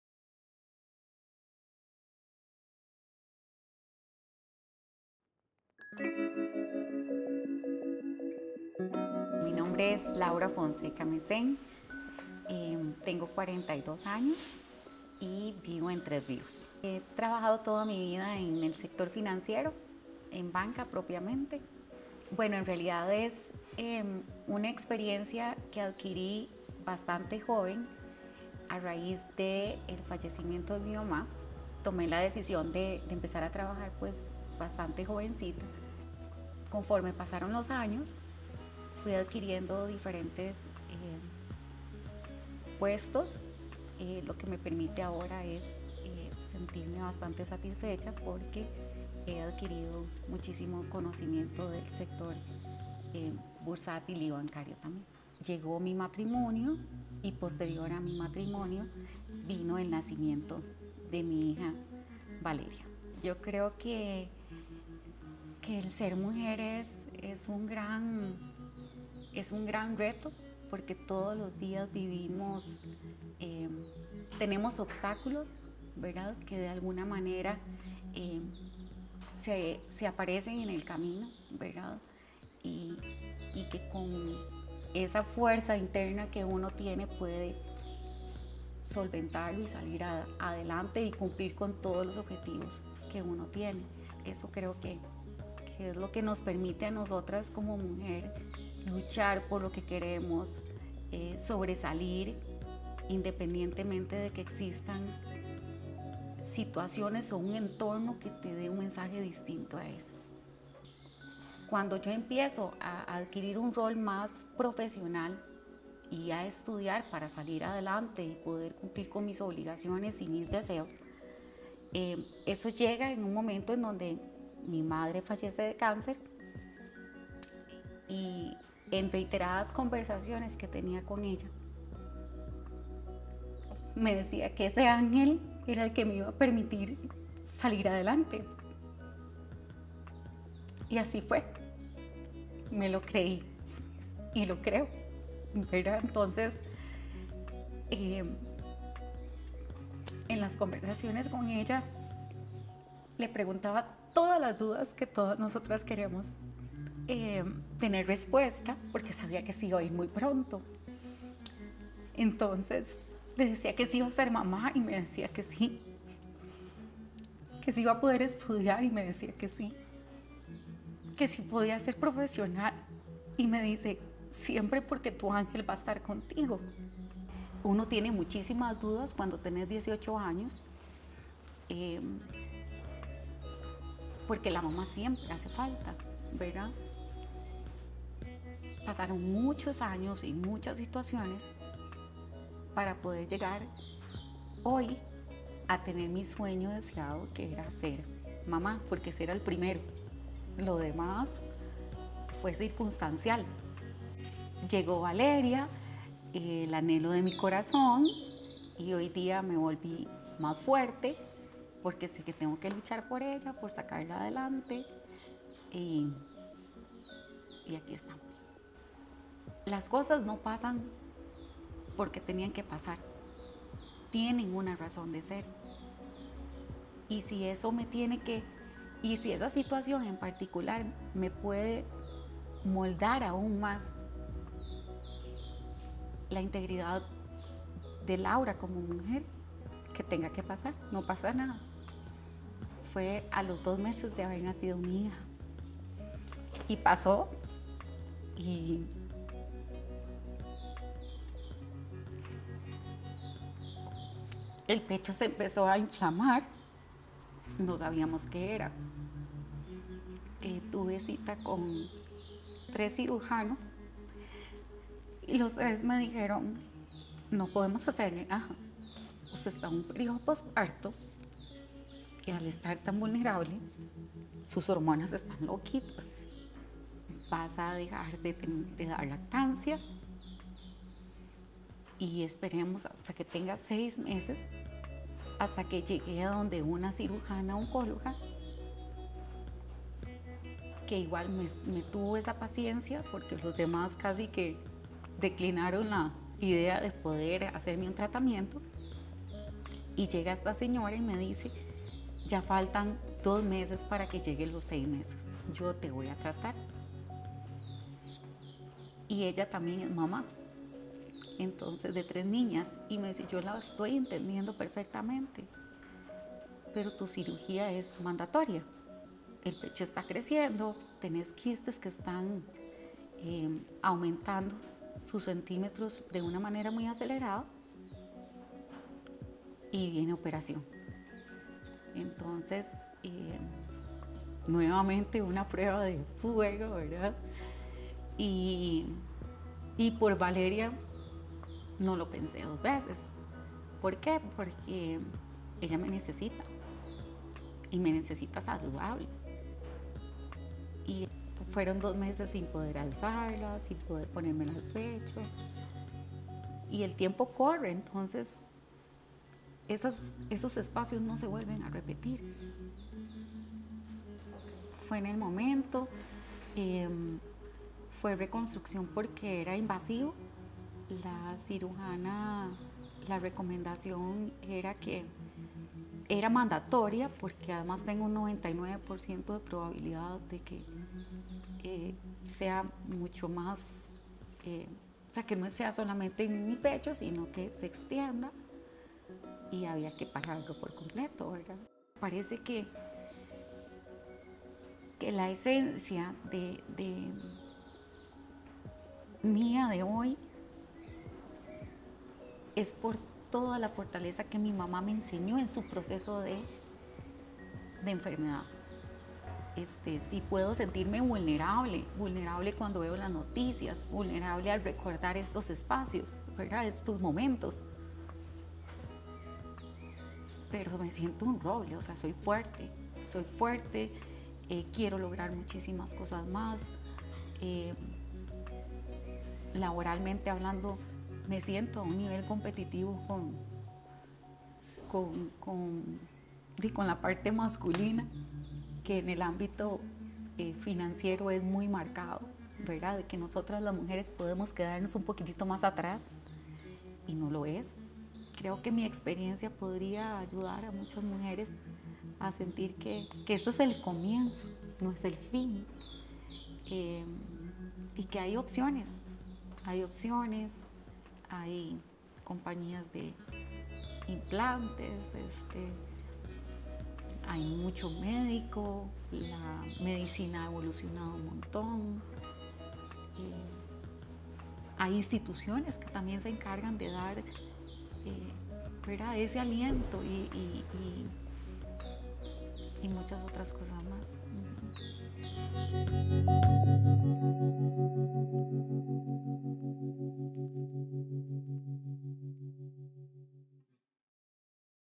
Laura - My breast recon story
Nothing could stop her from living her biggest dream, becoming a mother. Not even cancer! This is Laura’s story. 💗 A journey about dreams, motherhood, angels, and breaking all expectations.

Ask your physician about your options on breast reconstruction.